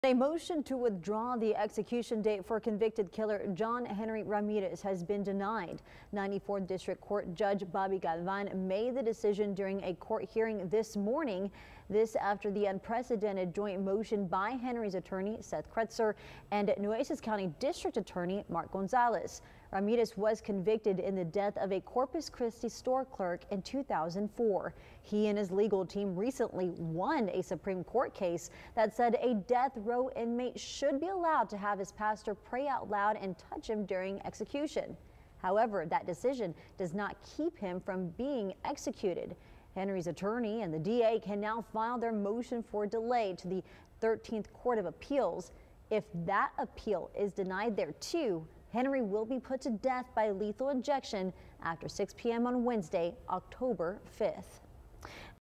DA's motion to withdraw execution date for John Henry Ramirez denied
In 2004, Ramirez was convicted in the death of a Corpus Christi store clerk. He is set to be put to death by lethal injection on Wednesday, Oct. 5.